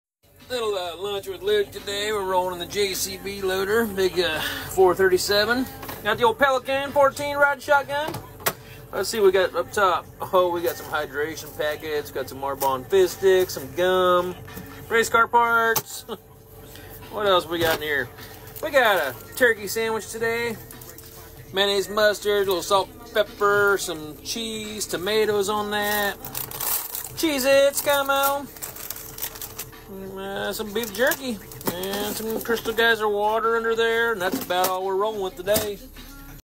BEST jobsite cooler!
The Pelican 14QT personal cooler is the best investment you'll ever make! It fits more than you would think!
You can find all Pelican Coolers at AdventureReady.com